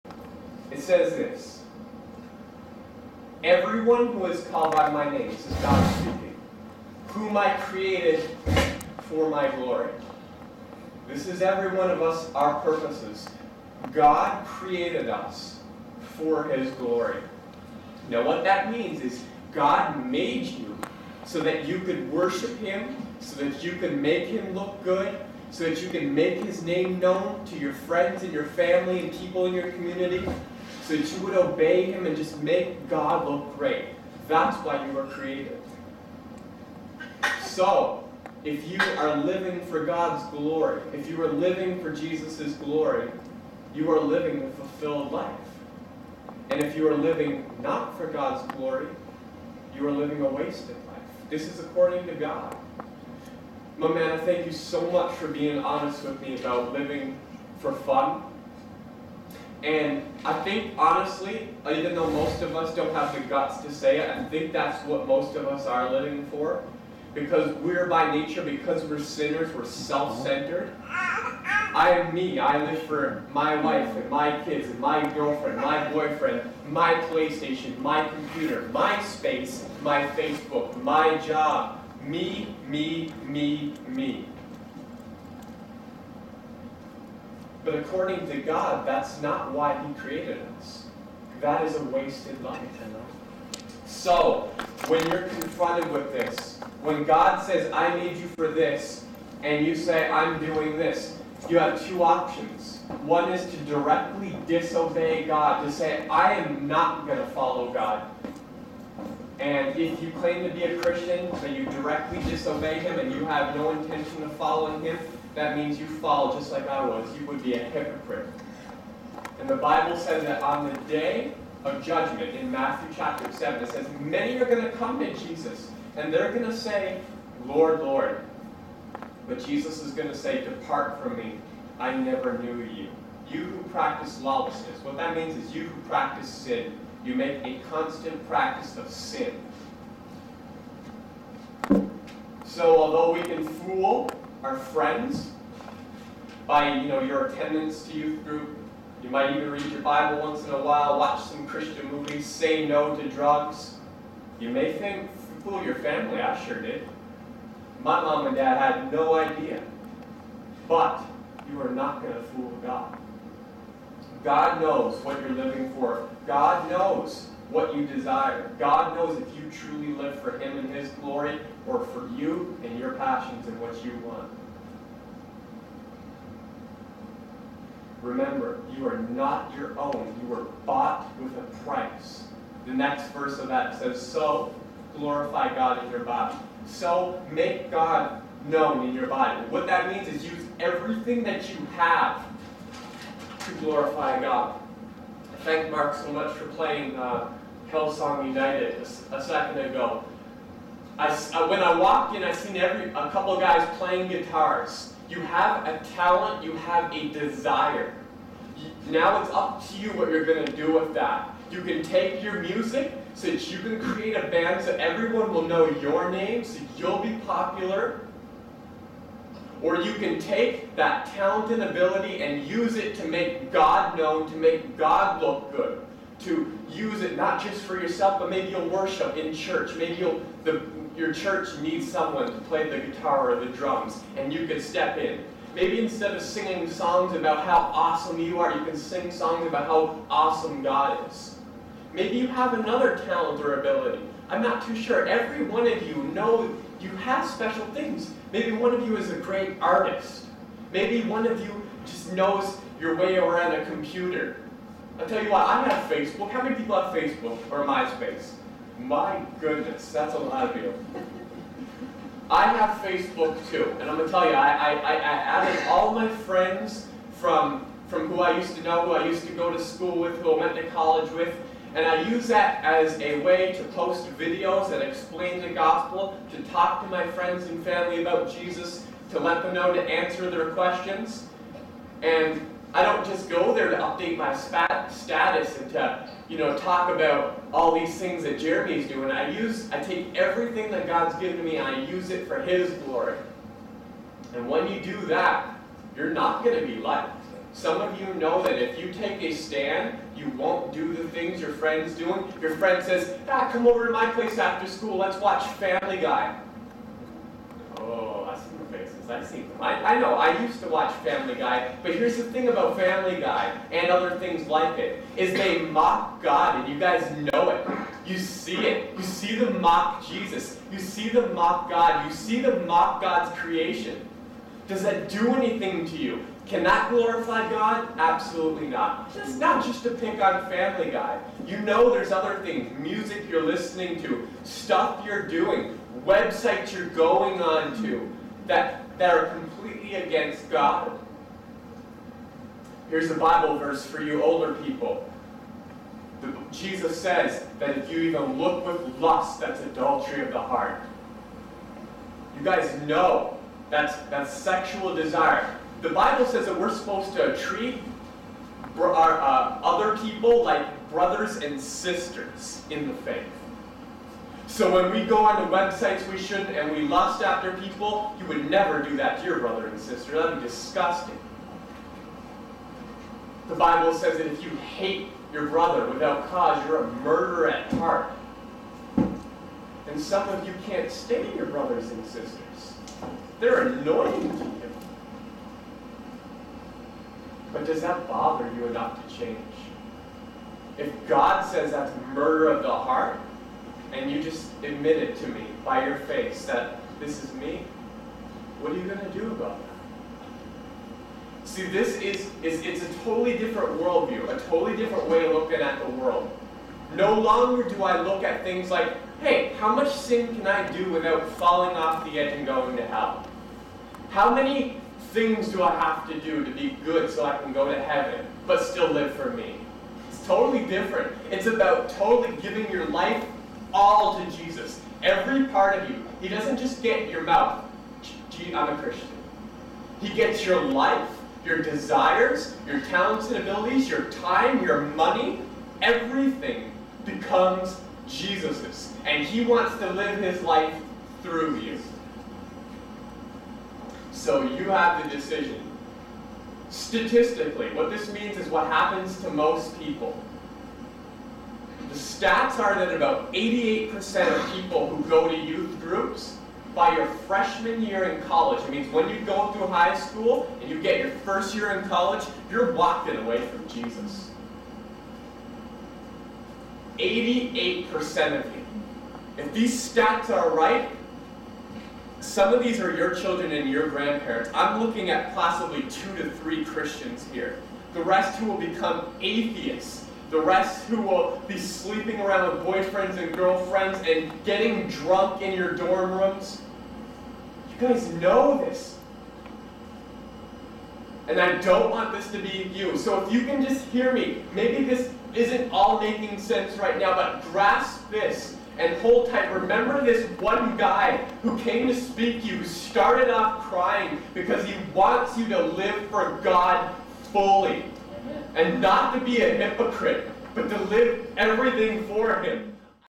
Don't be a Hypocrite. Don't Waste Your Life
Jeremy pleads with the Ethelbert Youth Group to repent of hypocrisy and embrace their life's purpose of glorifying God.